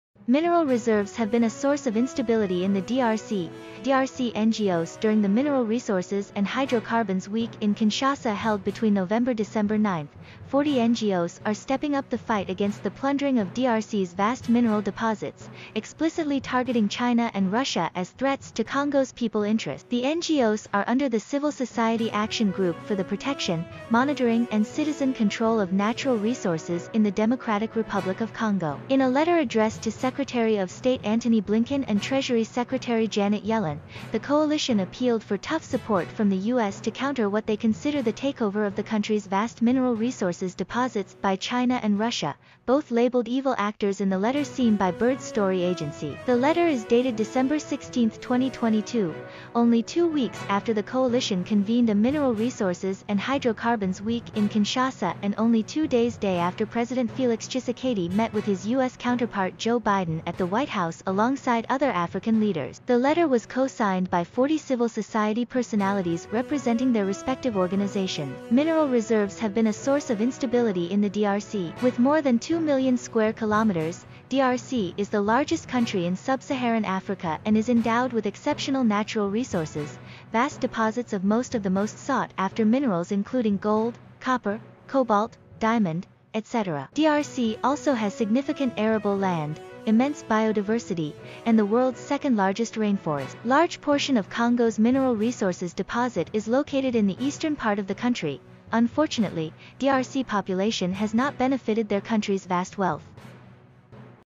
DRC: NGOs Accuse China and Russia of Resource Plundering, Seek US Support
Mineral reserves have been a source of instability in the DRC.